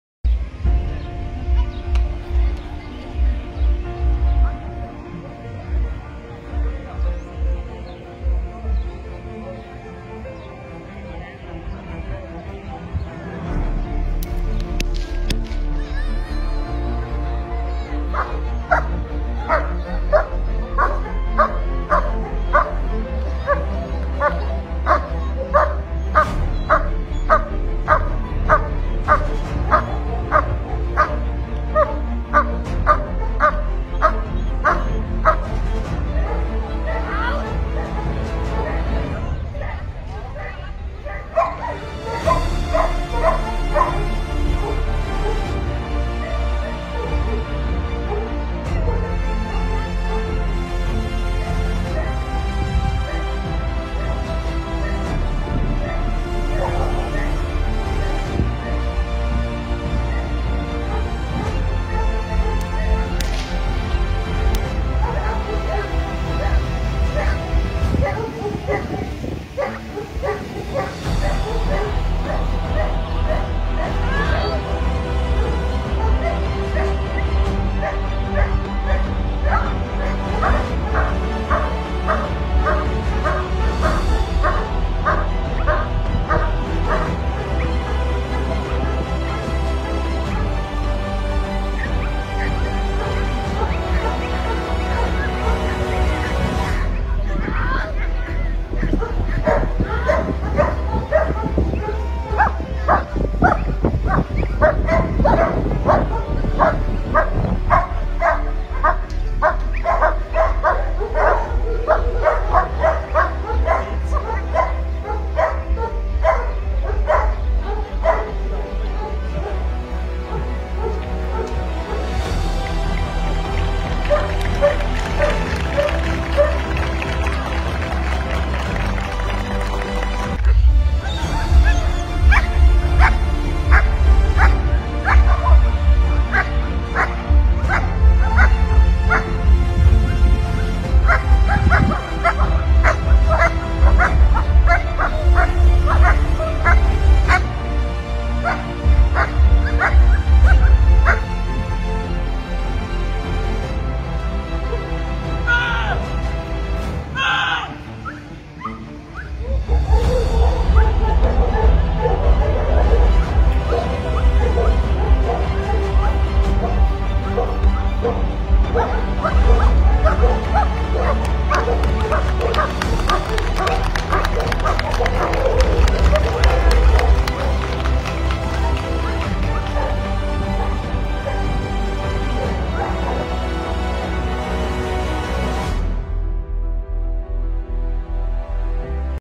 Davi de Apenas 8 Anos conduzindo um filho do nosso Axel. Pastor Alemão Amizade e Proteção.
Olha que legal esse filho do Axel conduzido pelo Davi de apenas 8 Anos e sua Mãe.
Esse é o Pastor Alemão!!!

XERIFF K9 do Paraíso das Águas
Pai: 4XVA Axel von Haus Zurück
Mãe: Angra do Rocha do Vale
Prioritário: Álvaro Leite Amazonas Júnior
Treinado por Canil&Escola K9-Sergipe
Criador: Isaias Nascimento Dos Santos Nascimento

Xeriff, Um cão de muito bom temperamento e muito bem treinado, Parabéns aos envolvidos.